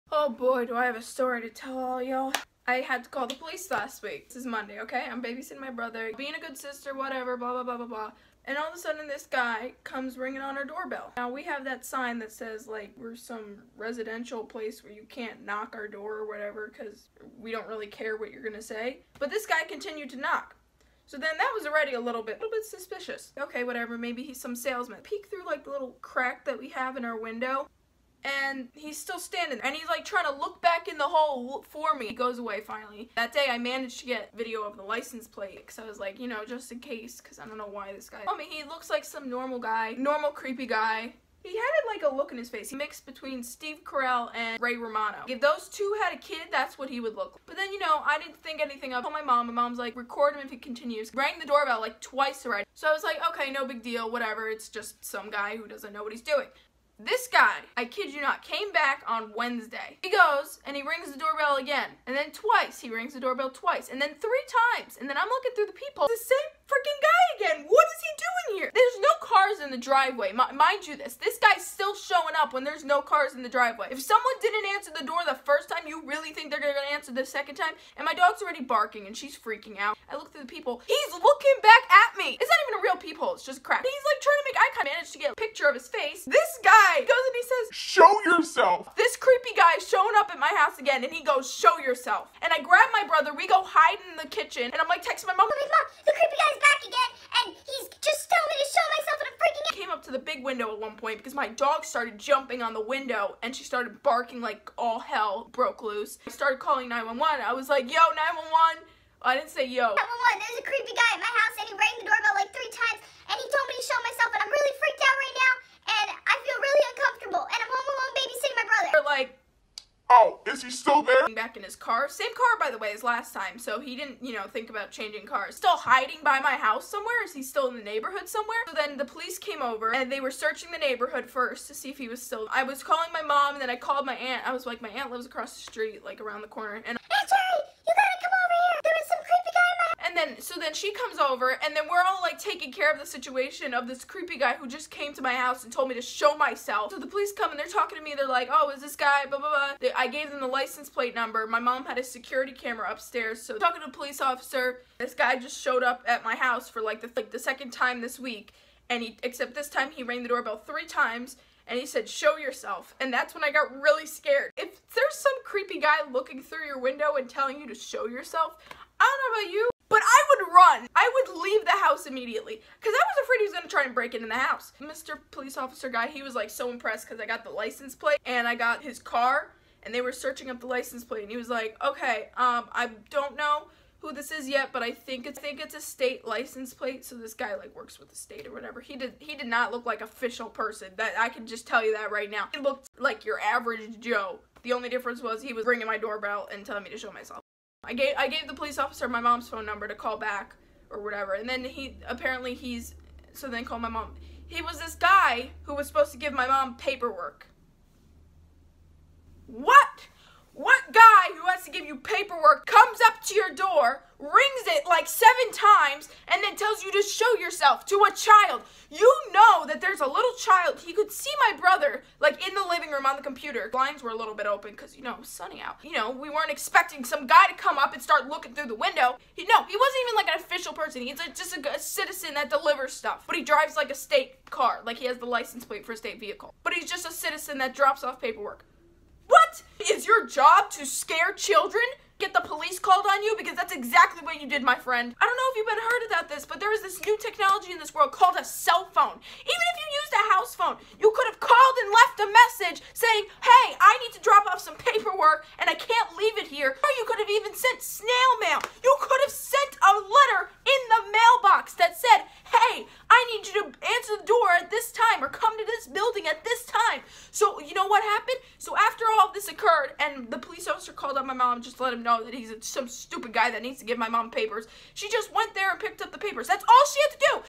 I Had to Call 911 | Rant 2
Not some stupid click bait!!! This happened a year ago, I just never posted it lol.

Extra:

Cameras I use:
iPhone 7